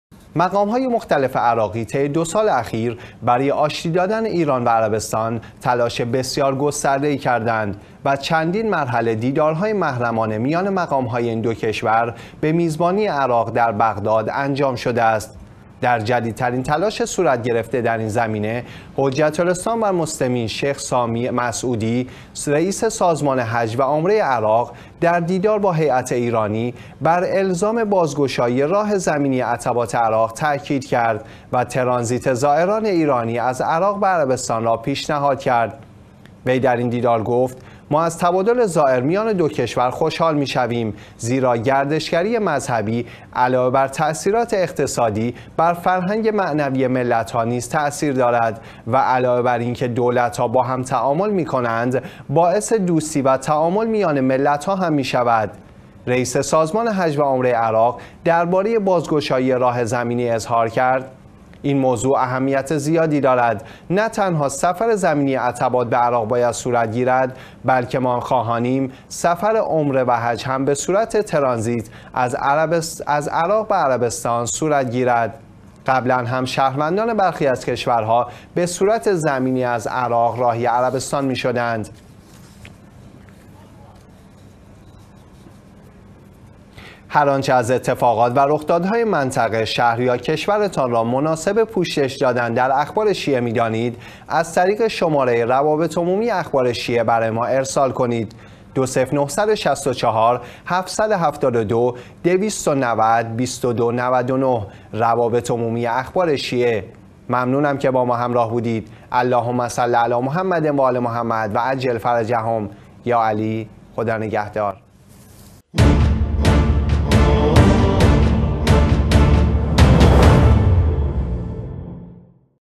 پيشنهاد رییس سازمان حج و عمره عراق:حجاج ایرانی از طريق مرز زمینی عراق به سفر حج مشرف شوند
مقام‌های مختلف عراقی، طی دو سال اخیر برای آشتی دادن ایران و عربستان، تلاش بسیار گسترده‌ای کرده اند و چندین مرحله دیدارهای محرمانه میان مقام‌های این دو کشور به میزبانی عراق، در بغداد انجام شده است.
در جدیدترین تلاش صورت گرفته در این زمینه، حجت الاسلام شیخ سامی مسعودی، رییس سازمان حج و عمره عراق در دیدار با هیات ایرانی، بر الزام بازگشایی راه زمینی عتبات عراق تاکید کرد و ترانزیت زائران ایرانی از عراق به عربستان را پیشنهاد کرد.
وی در این دیدار گفت: ما از تبادل زائر میان دو کشور خوشحال می‌شویم؛ زیرا گردشگری مذهبی علاوه ‌بر تاثیرات اقتصادی، بر فرهنگ معنوی ملت‌ها نیز تاثیر دارد و علاوه‌بر این‌که دولت‌ها با هم تعامل می‌کنند، باعث دوستی و تعامل میان ملت‌ها هم می‌شود.
رییس سازمان حج و عمره عراق درباره بازگشایی راه زمینی، اظهار کرد: این موضوع اهمیت زیادی دارد، نه تنها سفر زمینی عتبات به عراق باید صورت گیرد، بلکه ما خواهانیم سفر عمره و حج هم به صورت ترانزیت از عراق به عربستان صورت گیرد.
قبلا هم شهروندان برخی از کشورها به صورت زمینی از عراق راهی عربستان می‌شدند.